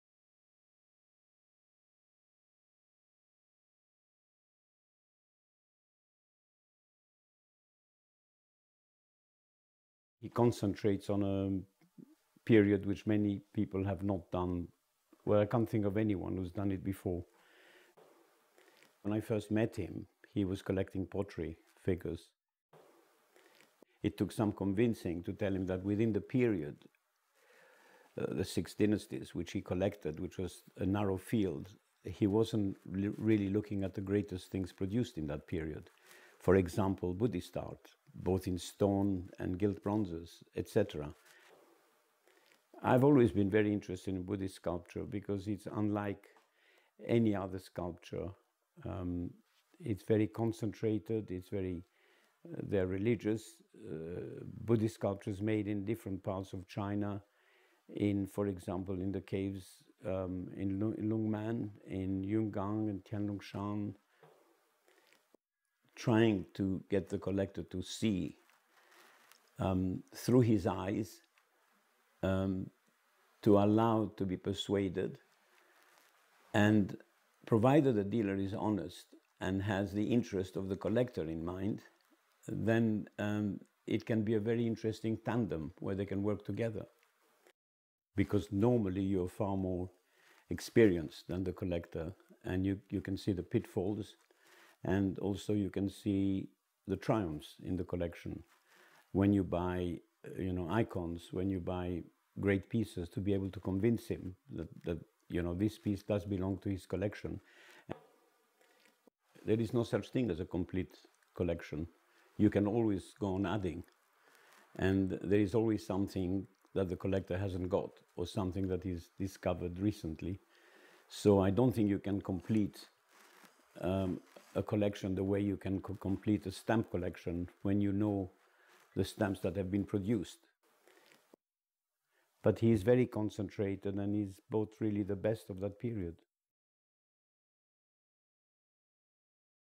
Giuseppe Eskenazi discussing the Norman A. Kurland collection of Six Dynasties Art.
Giuseppe Eskenazi highlights some of the key points in the collaboration between dealer and collector in the forming of a great collection.